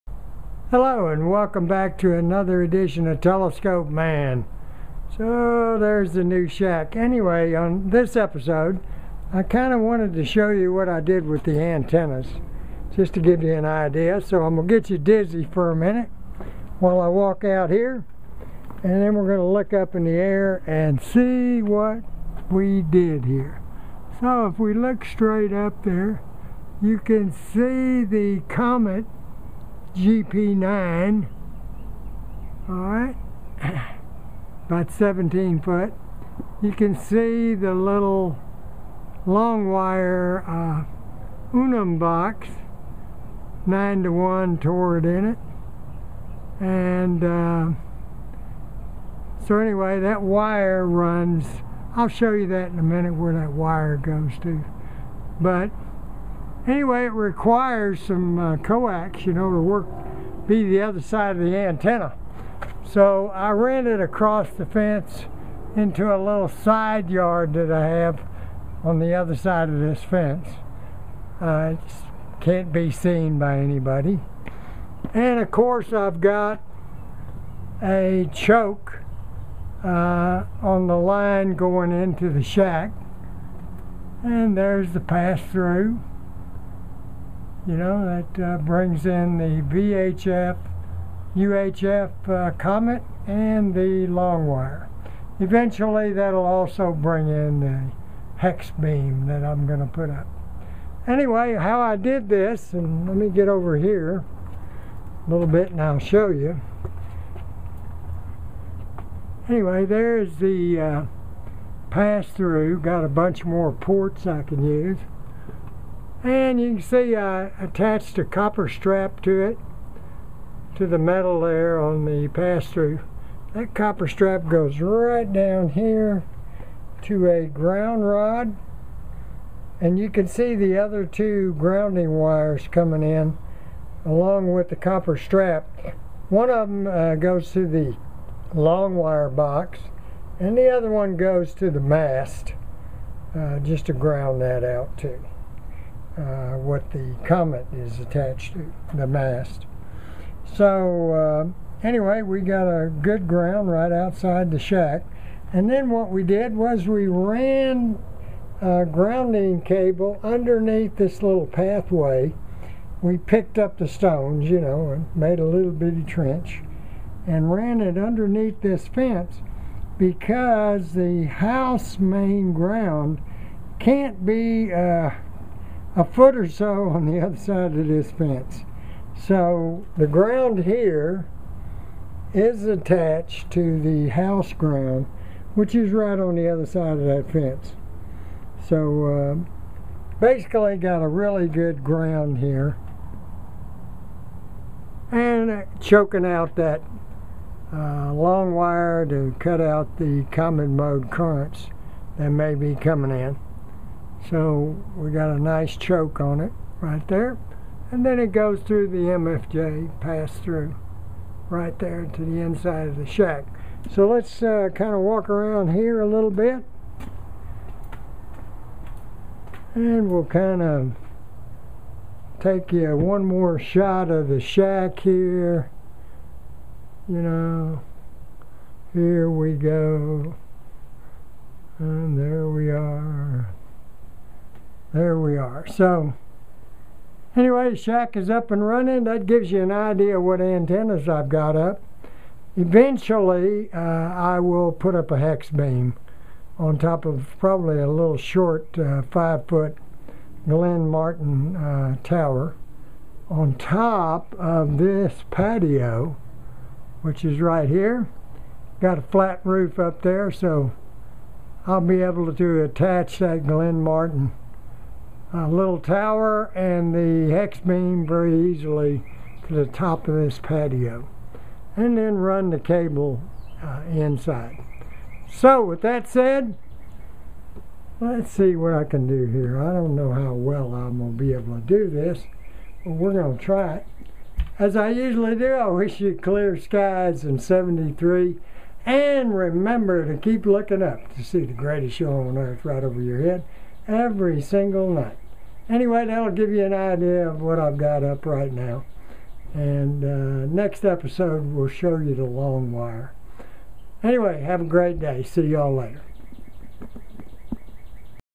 New ham shack antennas and grounding
TelescopeMan shows his two antennas at the new ham shack along with the grounding arrangement.